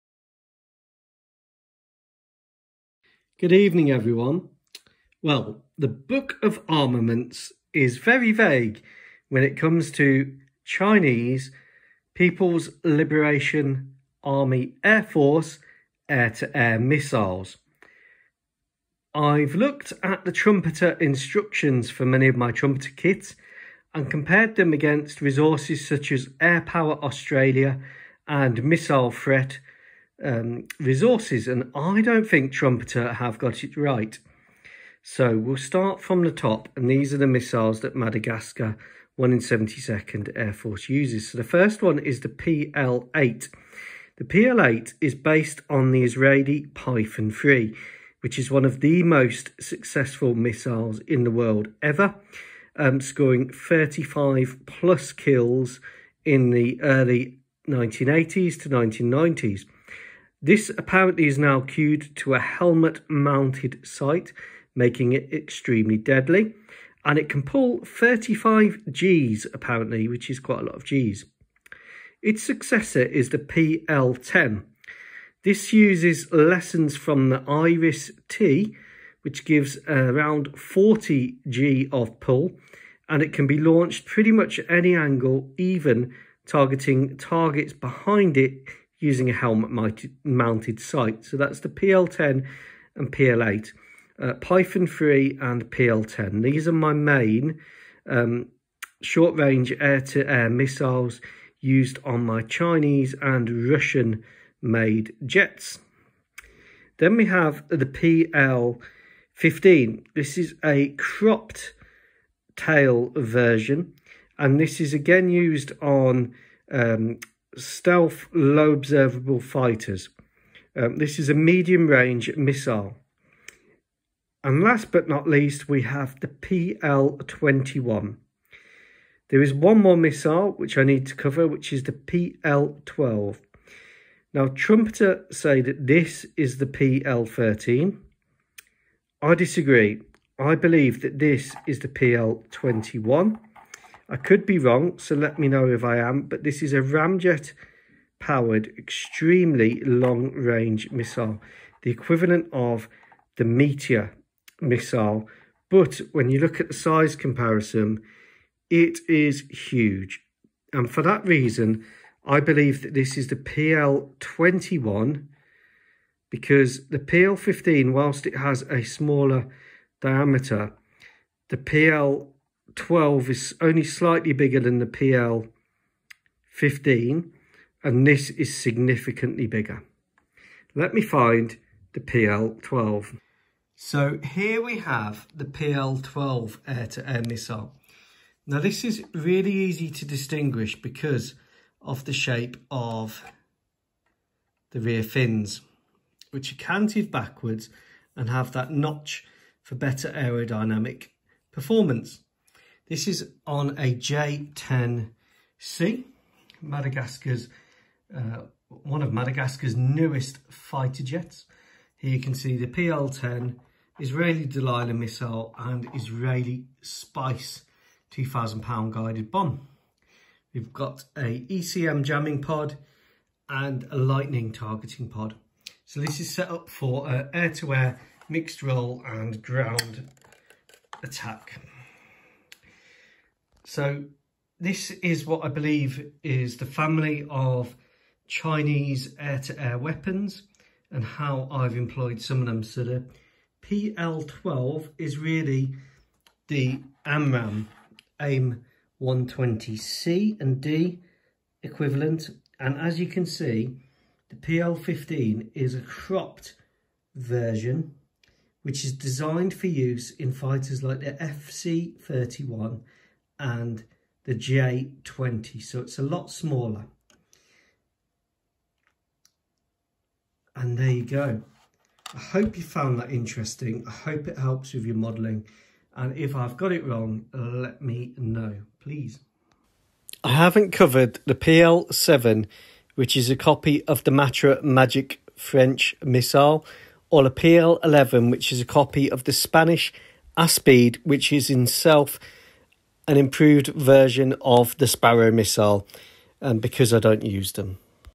1/72 Book of Armaments. Chinese PLAAF Air to Air Missiles - to assist with model aircraft kit build
Pl-8
Pl-10
Pl-12
Pl-21
Pl-15

I haven’t covered the PL-7 or 11 as I don’t use these and they are basically licensed productions of somewhat obsolete missiles!

I am convinced what Trumpeter market as the PL-13 is the PL-21 but I’m ready to be corrected :)

I think that it is the PL-21 due to its size!  The PL-13 (or PL-12D) is meant to be the equivalent to the Meteor missile.  What Trumpeter have modelled is significantly larger.  As the PL-21 is significantly larger and is designed as an extreme long range missiles I think that this is therefore more likely to be it.

Finding details on Chinese missiles is really hard!